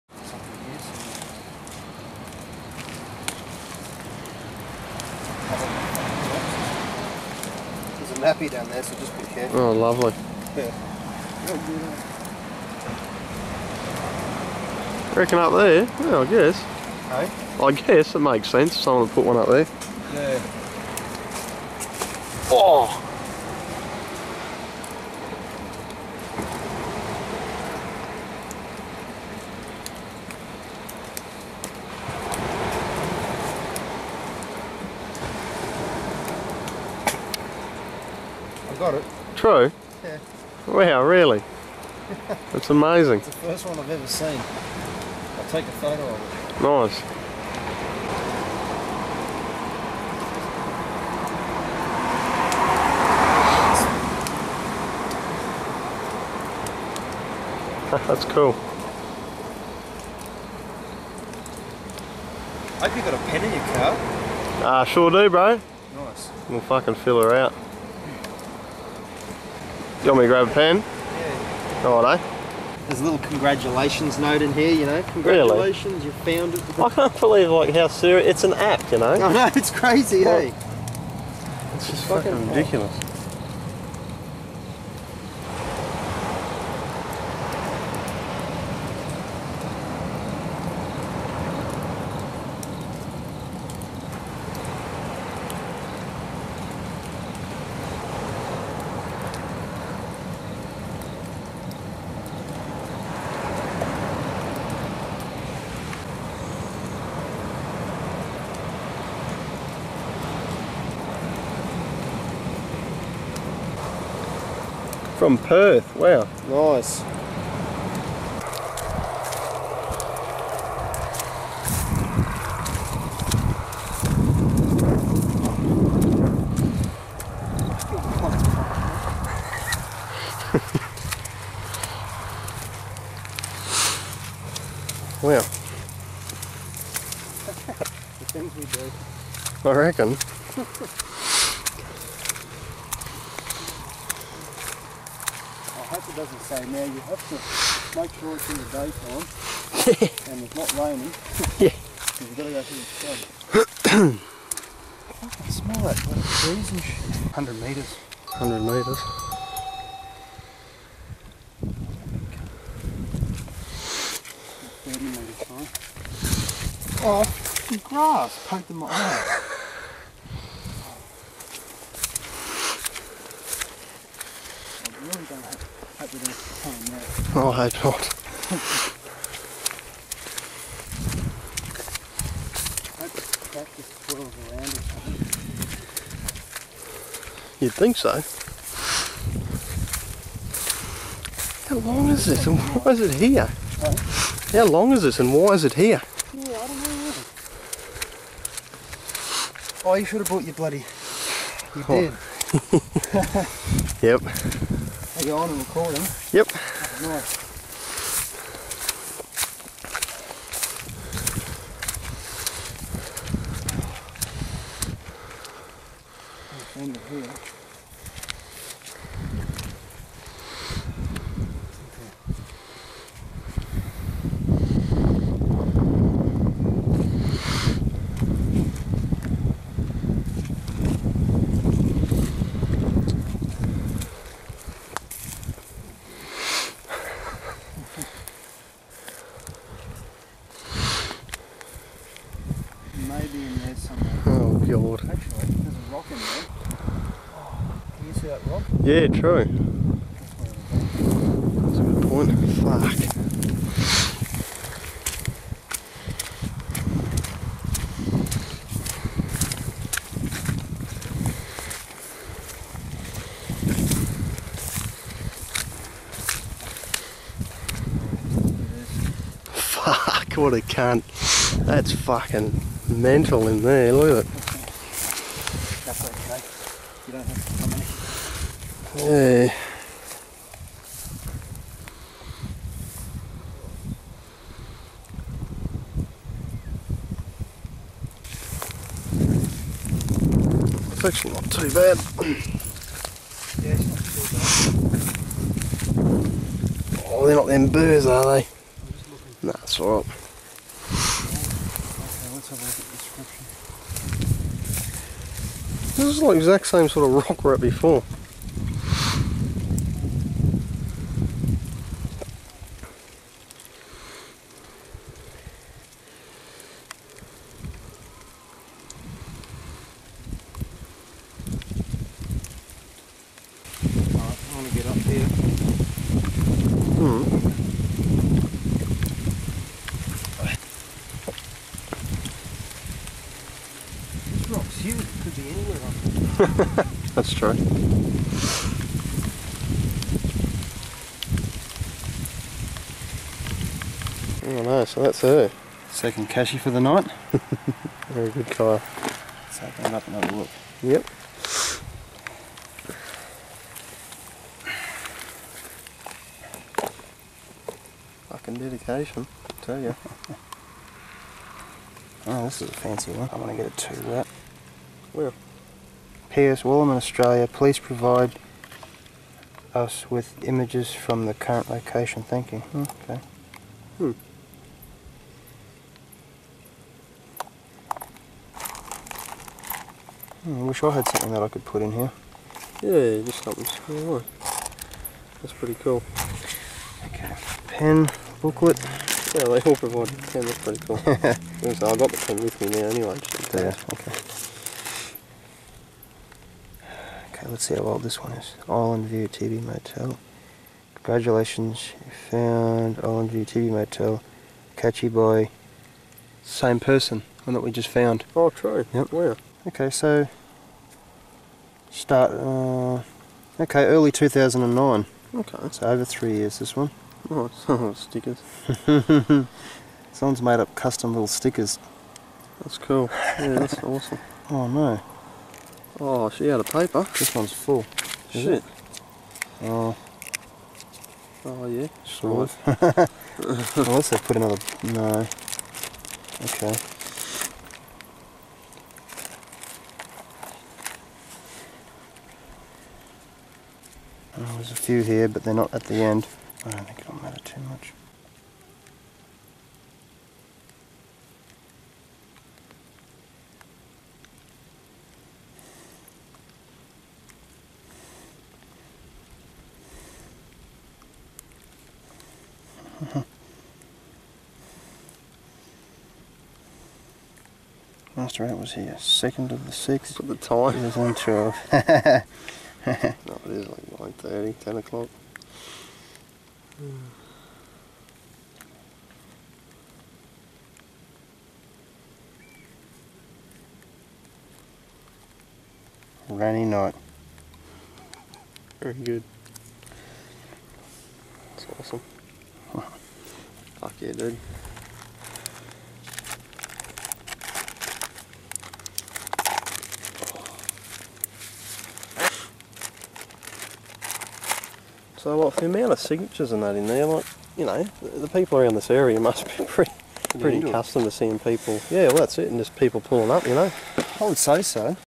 Geo Caching Quest - Episode I [HD]
Hey guys here's something a little different, my good mate put me on to this whole geo caching thing on the smart phones and at first i honestly thought it was all bullshit but it turned out to be real and quite interesting, so here's episode 1, more to come so enjoy, stay tuned and SUBSCRIBE!